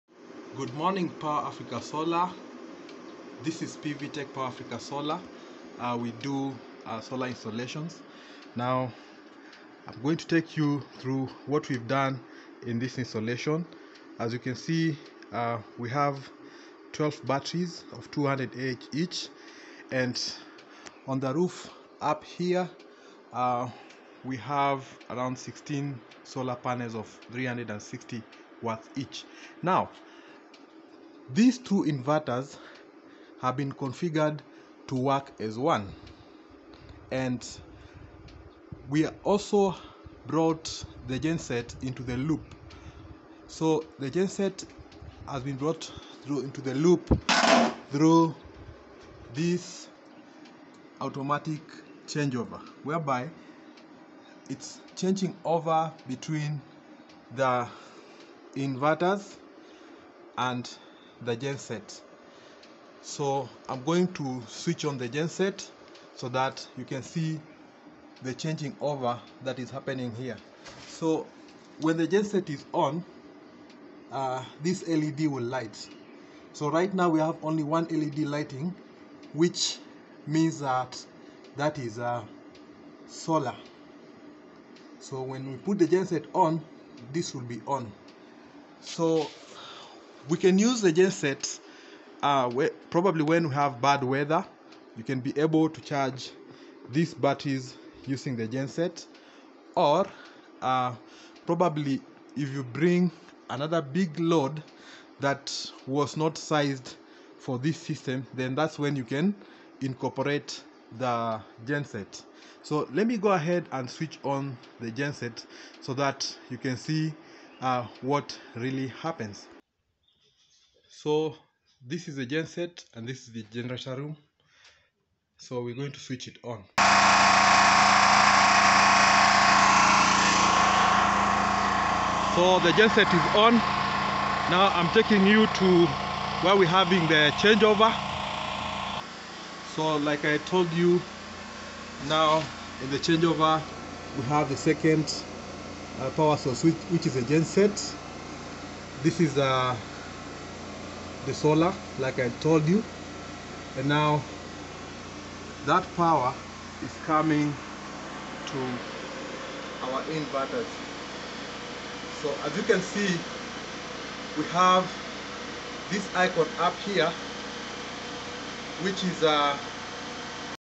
What You Can Power With a 6KW (6KVA) Solar System in Kenya
What You Can Power With a Scalable 6KW (6KVA) Solar System in Kenya

6 Bedroom FARM HOUSE With 5.76 Kw Scalable OFF GRID Solar System & Multiple Power Sources in Kenya